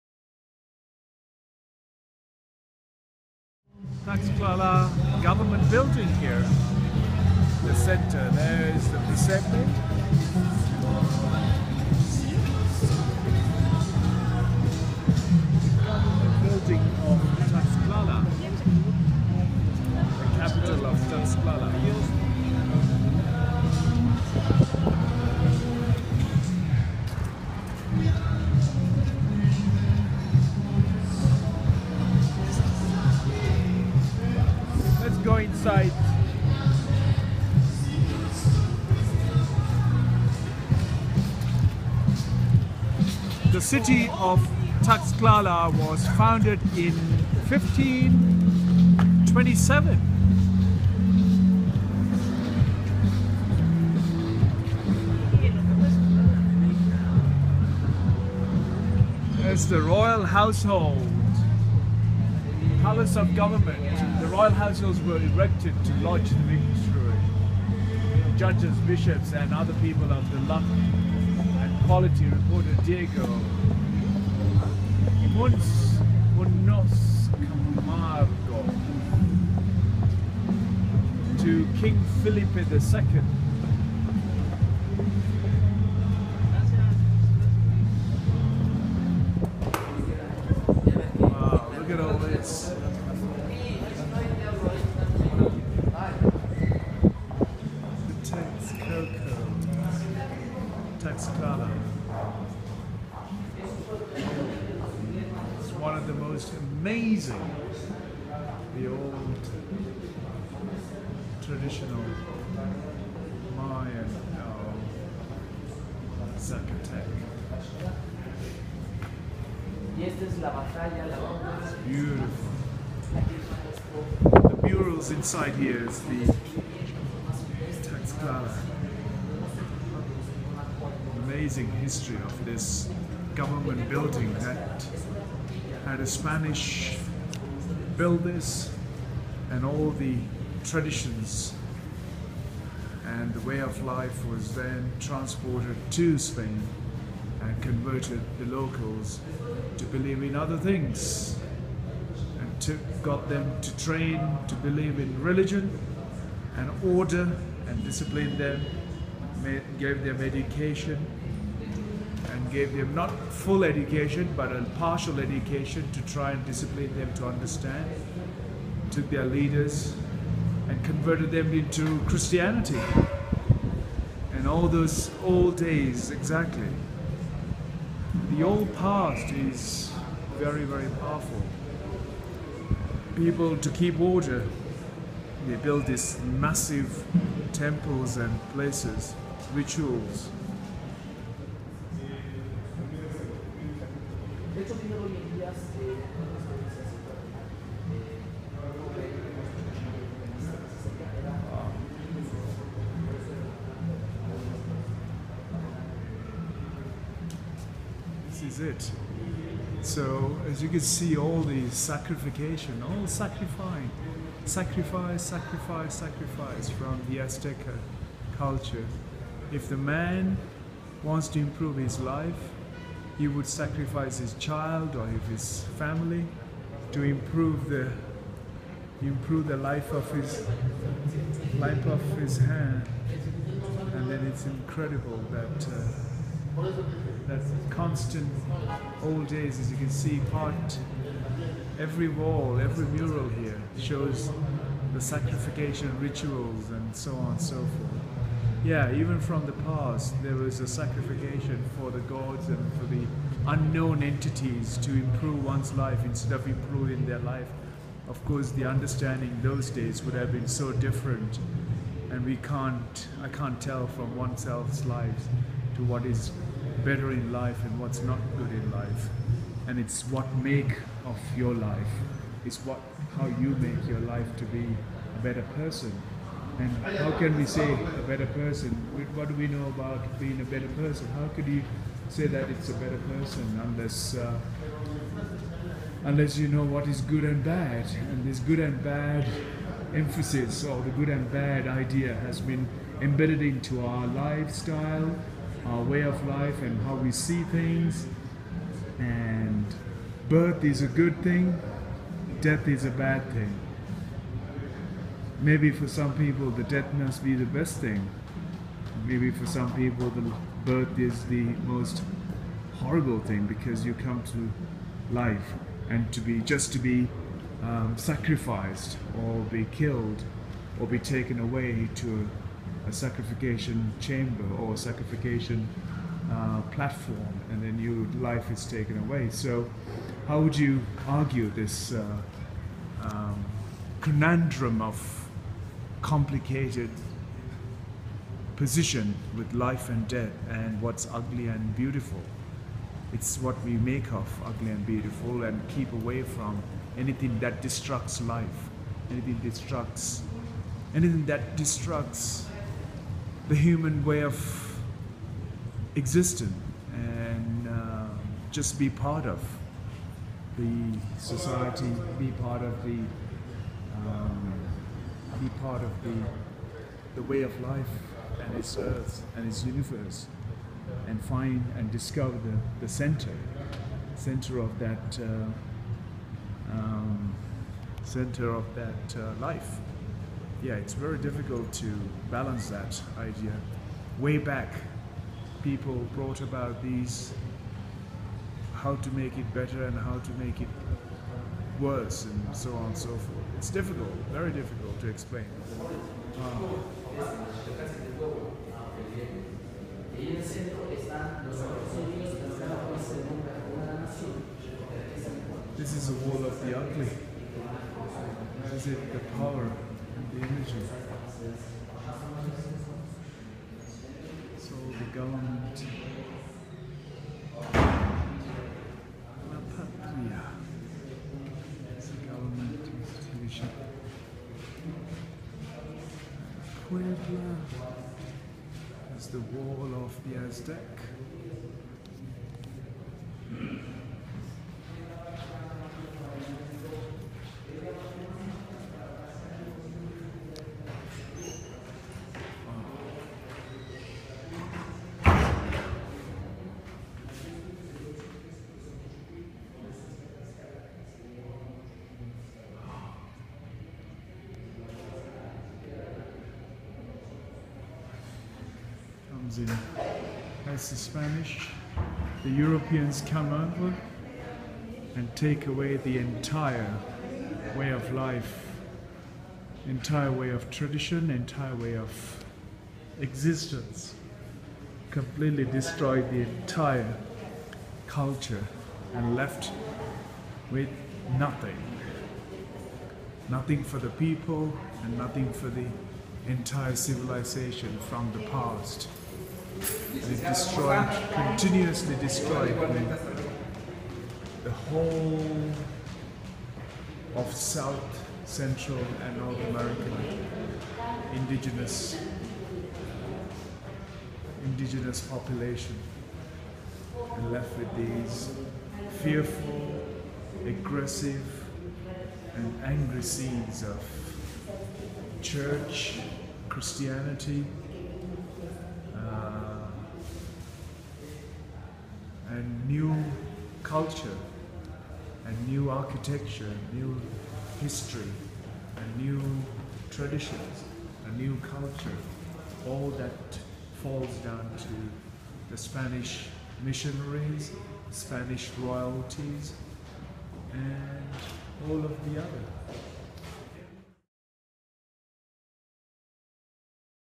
Palacio Municipal Tlaxcala Mexico
The Palacio Municipal was begun in 1550. the Palacio de Gobierno was begun in 1545.

This building was a grain storehouse.

Tlaxcala is the stunning city and is located in Mexico’s central eastern region, 120 kilometers east of Mexico City. The climate is mild, So much history and the paintings in side of the Palacio is impressive.